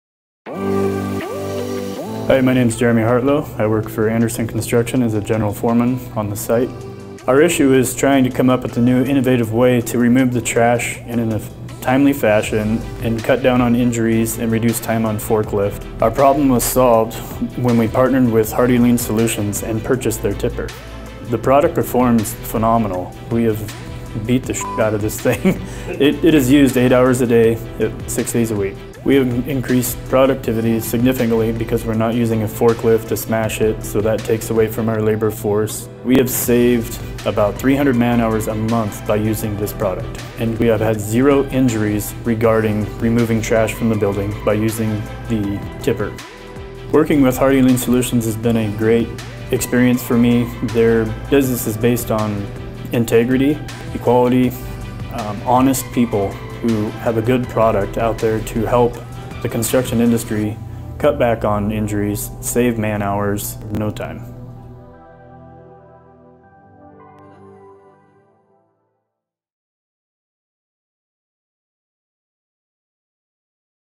HardyLifts HLS Anderson Construction Testimonial
Hey y’all…

Did you know, back injury is the top cause of a ‘job-related disability’ and more than one million back injuries are sustained in the workplace annually?

Additionally, back pain accounts for more than 264 million lost workdays costing around $225.8 billion in lost productivity annually and in the neighborhood of $50 billion just treating employee back pain.

Introducing HardyLifts Tipper, a battery-powered mobile trash tipping unit that lifts up to 1,000lbs and has been proven to reduce injuries and increase productivity throughout many industries.

Hardy Lean Solutions takes pride in providing equipment and solutions that focuses on safety and efficiency.

So, if you are looking for a simple solution that could potentially save you tons of money and your employees a world of pain and suffering, check these guys out and tell me what you think.